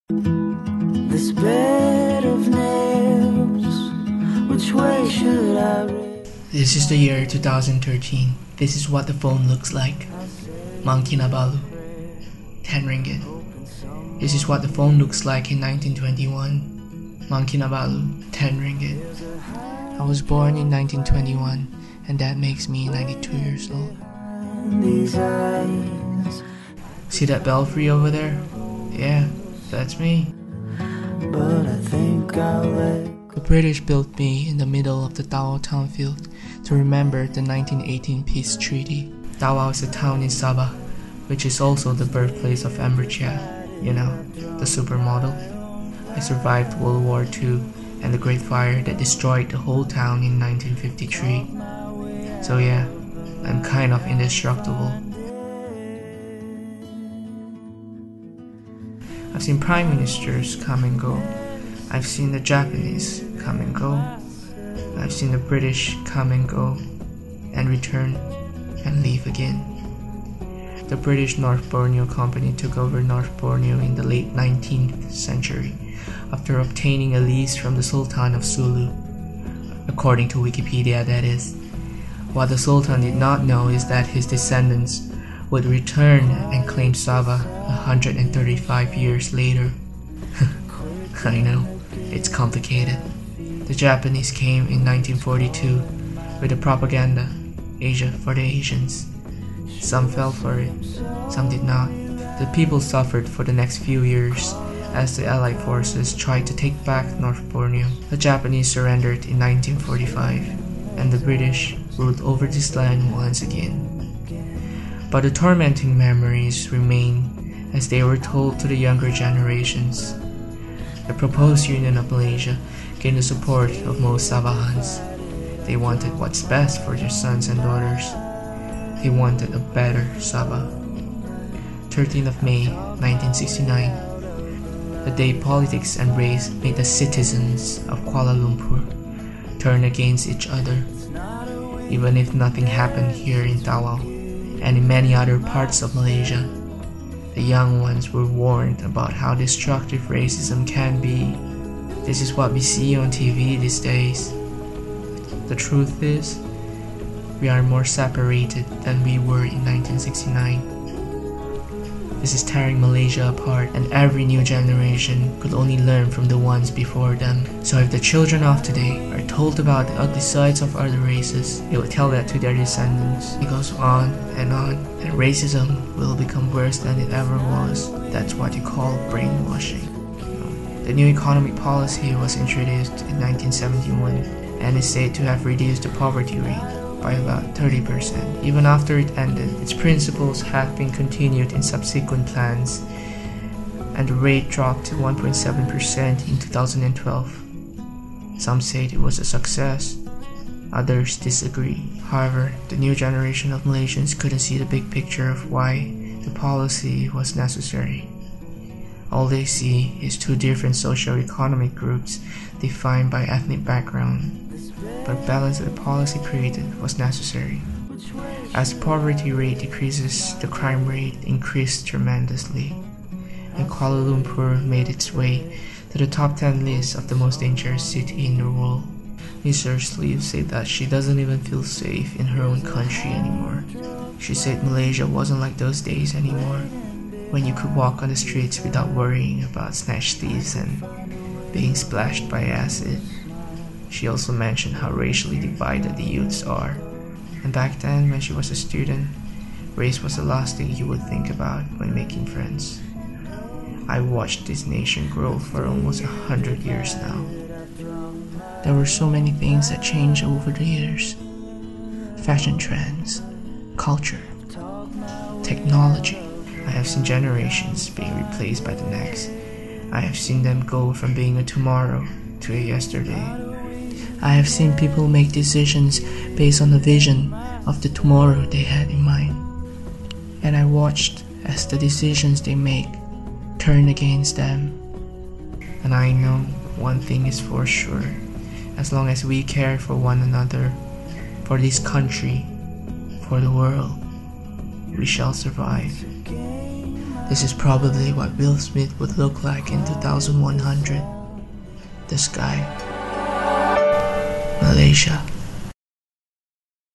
Belfry by Cephas Liew (UKEC MSLS Video Competition)
A Short Film Entry for the UKEC MSLS Video Competition 2013

To match the theme, 'Malaysia : Tomorrow in the Eyes of Yesterday', I wanted to showcase the transition of a generation from being the future of a country to the past. Therefore, I used the belfry because it has been there for about 4 to 5 generations of Malaysians and has witnessed the transitions. I also wanted to show how people make decisions based on the future they want to see and how it actually turns out. The picture of Malaysia is a blank screen because it is up to the viewers to create their own vision of a future Malaysia. If they want a better Malaysia, then it is up to them to do something about it. Most of the topics are based on familiar past events because I am hoping that the viewers would be able to easily understand the values that I am trying to convey through this video. I used Will Smith to exaggerate the fact that people say he doesn't age at all which makes his looks predictable in 2100, just like the sky